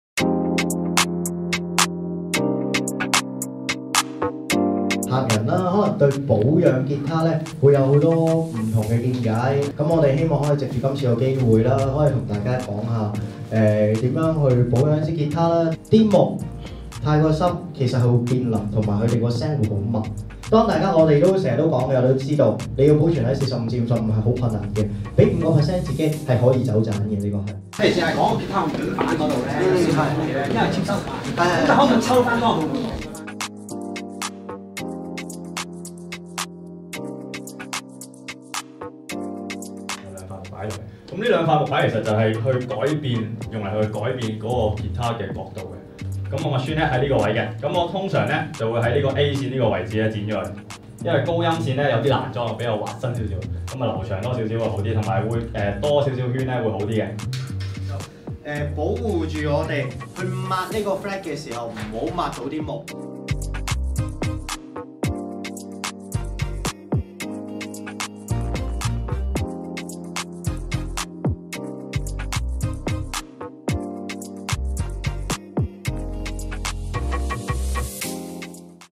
【通利琴行 — 結他保養工作坊】
早前舉行嘅結他保養工作坊已經圓滿結束啦～
當日分享咗啲大家常問嘅問題，一齊去片睇吓當日嘅活動回顧啦！
如果有任何查詢，都可以嚟到通利琴行問問店員㗎！